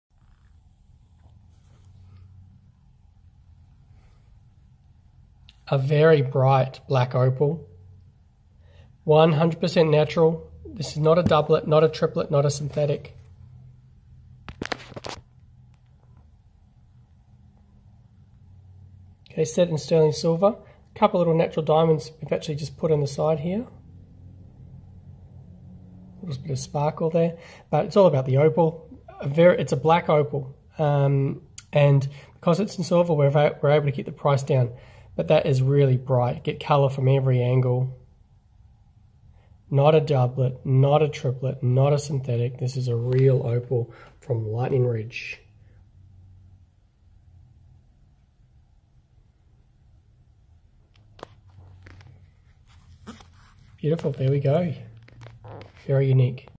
Sterling Silver Ring from Brisbane
Very Bright Black Opal Ring - Sterling Silver with Diamonds it has real natural opal stone, from Lightning Ridge, very unique piece of jewellery, green is the popular colour on stone. For Natural Opal Jewellery from the website in the featured video, please go For information about opal or visit the Opal Museum in Brisbane Australia go